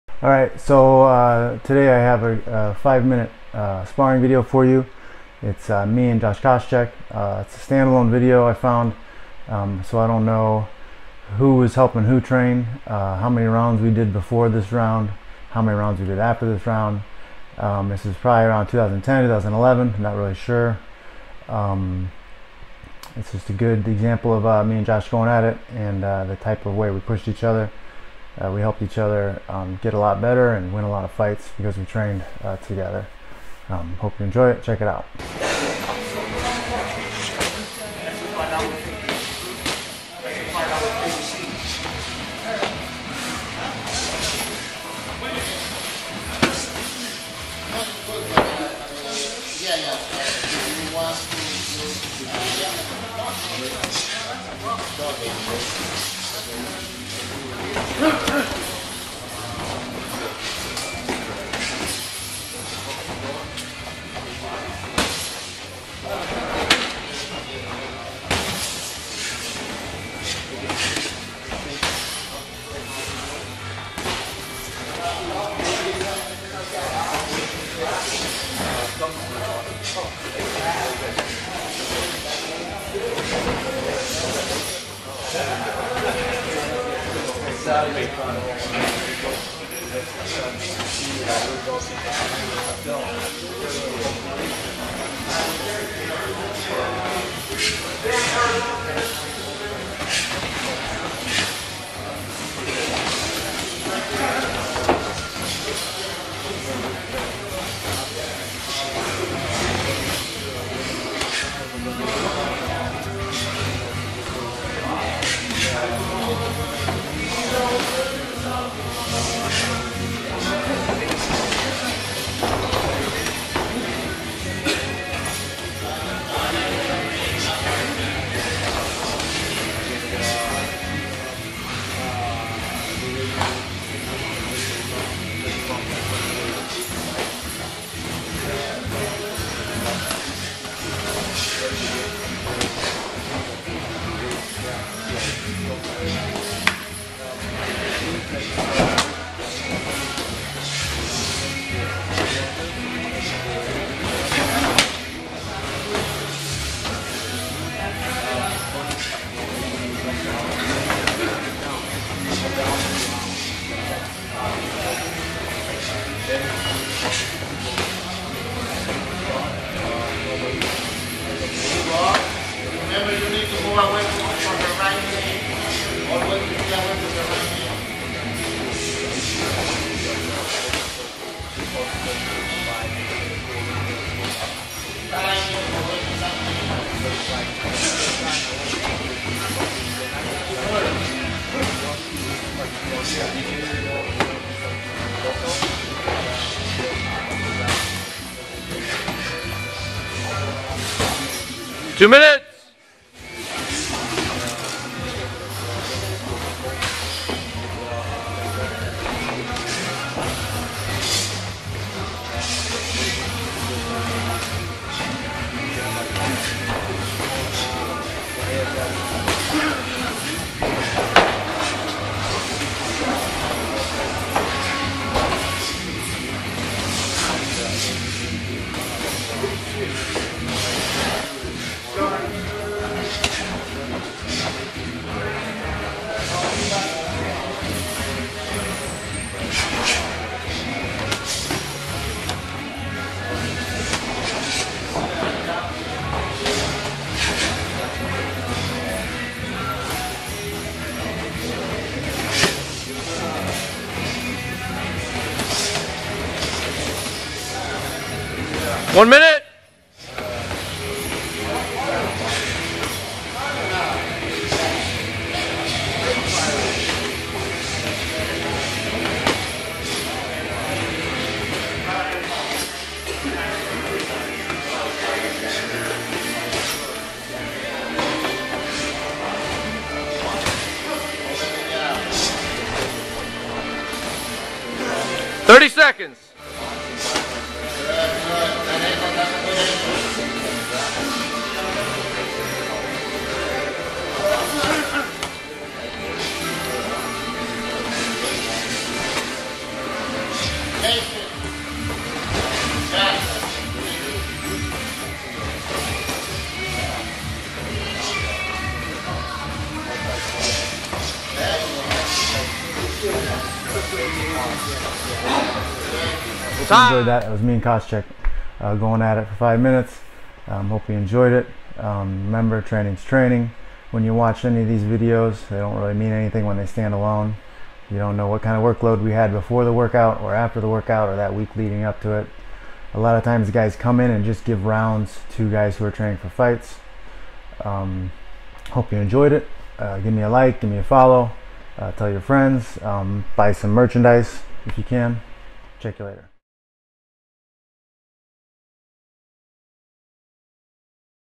Jon Fitch Lost Tapes 11
This is an old video of Josh Koscheck and me doing some hard sparring. This is from 2010 or 2011. Its a 5 min round and I'm not sure what we were training for. its mostly us sparring on our feet. When 2 good wrestlers fight the fight can end up entirely on the feet. Enjoy